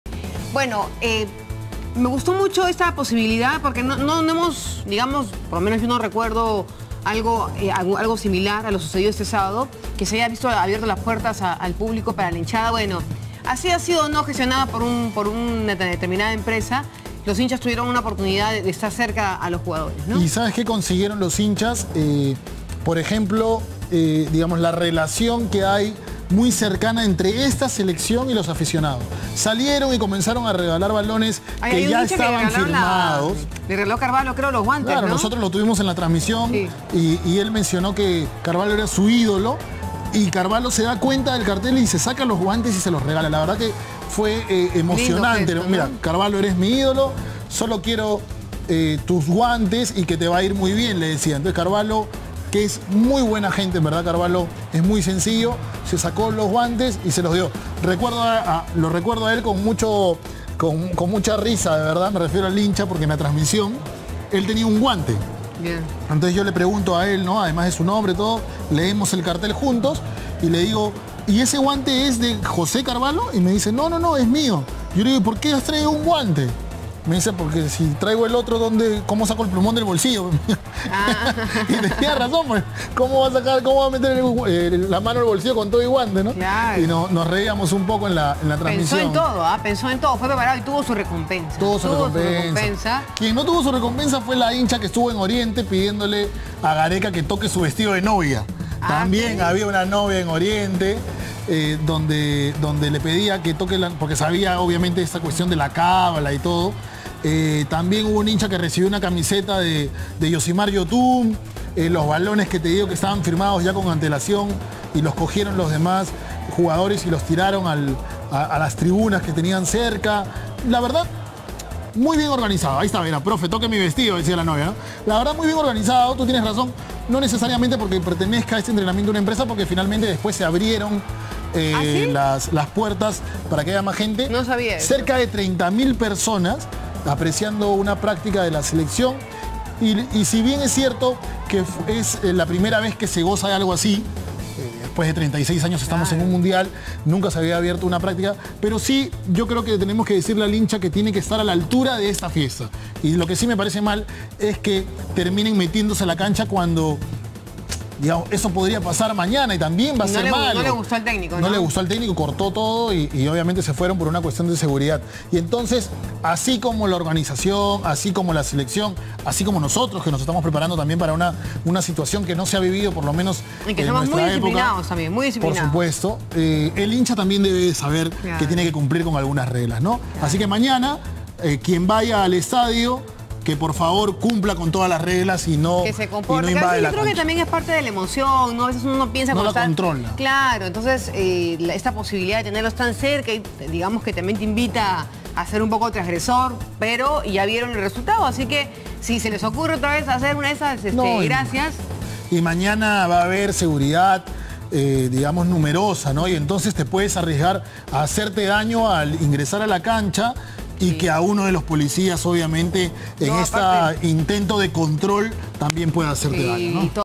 José Carvallo y el gran gesto que tuvo con un hincha en el entrenamiento
Al final del 'entrenamieeto de despedida', el tercer portero de la bicolor, José Carvallo se acercó hasta la tribuna occidente y entregó su par de guantes a un aficionado, quien lo llamaba desde varios metros de distancia.

El hincha recibió el inesperado regalo y su alegría desbordó tanto así que los demás asistentes no podían creerlo. Carvallo se retiró caminando y saludó a todos los asistentes de esa parte del coloso de José Díaz.

Esta sorpresa y otras más se presenciaron durante varias partes del entrenamiento de la selección peruana, donde al final de la práctica, varios hinchas saltaron a la cancha para tomarse un selfie o pedir un autógrafo.